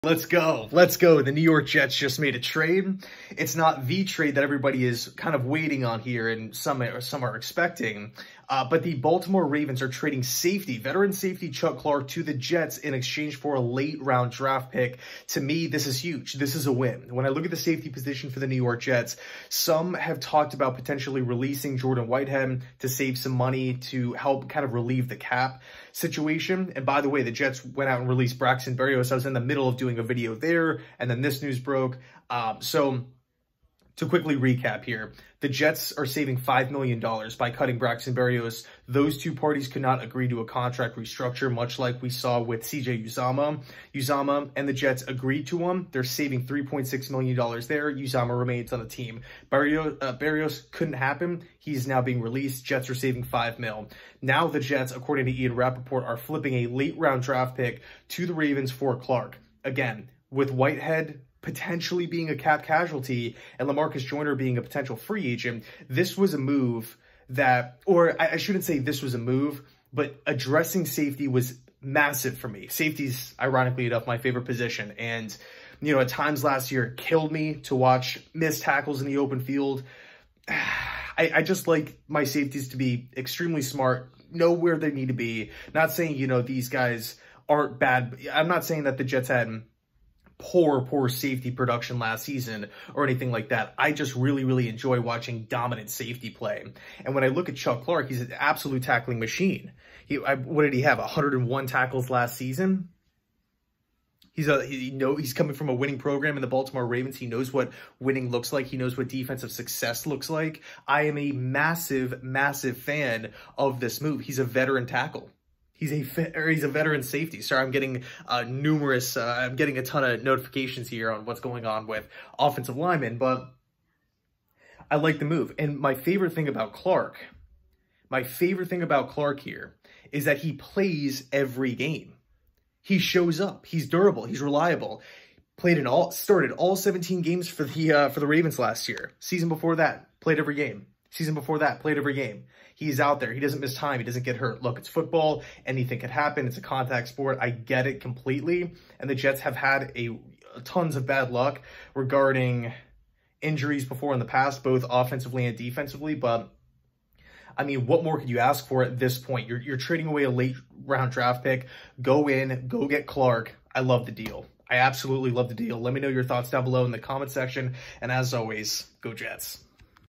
BREAKING:New York Jets ACQUIRE Chuck Clark From The Baltimore Ravens! | Release Braxton Berrios
The New York Jets are acquiring Safety from the Baltimore Ravens Chuck CLark! Huge news as safety was a massive ned for the team and Clark will be coming in as veteran! The NY Jets have also went out and released WR Braxton Berrios saving $5million against the cap! Is Aaron Rodgers on his way next?

Ian Rapoport: "Sources: The Ravens are expected to trade safety Chuck Clark to the Jets in exchange for late-round draft pick compensation. The starter since 2019, Clark now adds veteran experience to NYJ. It should be official when the league year begins."

Adam Schefter: "Trade: Jets agreed to send a 2024 seventh-round pick to the Ravens in exchange for safety Chuck Clark, per league sources. Trade is agreed to, but cannot be processed until new league year begins Wednesday."

Aaron Rodgers - New York Jets Latest | Jets Are "Optimistic They Are On The Brink" Of Landing Aaron